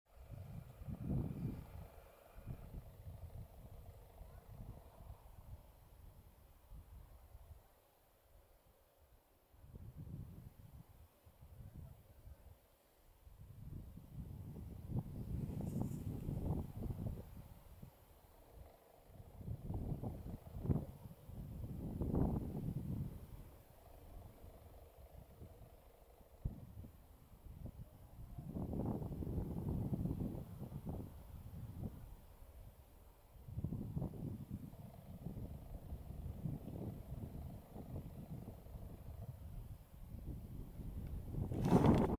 Korhanen op de Sallandse Heuvelrug (Black grouse)
Here are two male from the Black grouse. The place called "Kleine Koningsbelt". It is a beautiful hill in the center from the heather. This is the last area in the Netherlands where they are. The last population, they are very rare birds. That's why it is very special. In the past there were many of them all over the country, but the problem is, the landscape is chanced the last 100 years. And also the chicks eat insects for the first two weeks of their lives, and there are too few of them, so most chicks don't survive. (februari 2020)